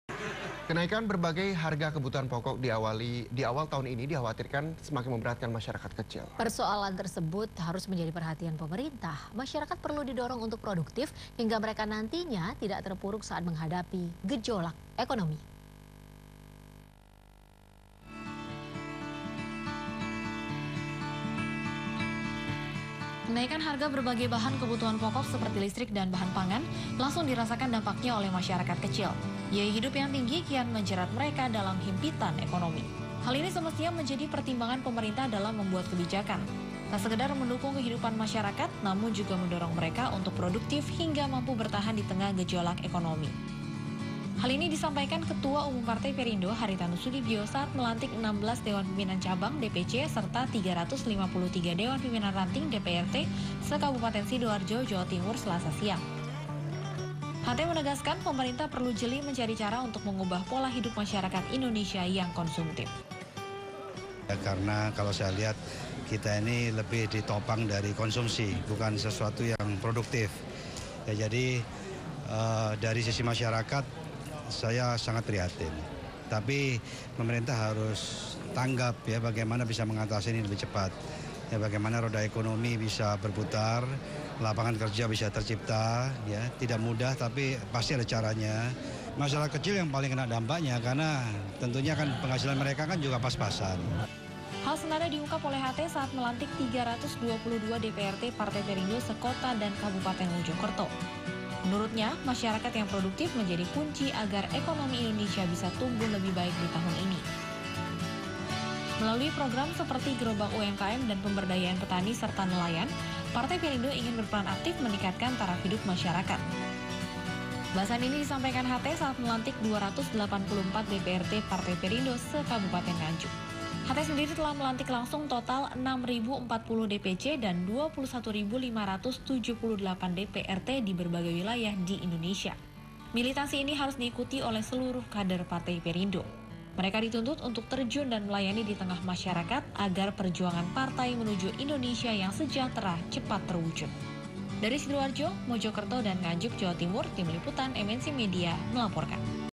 Hary Tanoe: Pemerintah harus Tanggap hadapi Lonjakan Harga
Kenaikan sejumlah berbagai kebutuhan pokok akhir-akhir ini harus disikapi serius oleh Pemerintah. Lonjakan harga yang tidak wajar seperti pada cabai harus segera diatasi. Masyarakat, terutama yang miskin, banyak dirugikan. Daya beli mereka yang tidak seberapa makin tertekan. Hal ini ditegaskan Ketua Umum Perindo Hary Tanoesoedibjo seusai melantik Pengurus DPC dan DPRt Perindo Sidoarjo, Jawa Timur, Selasa (10/1/2017)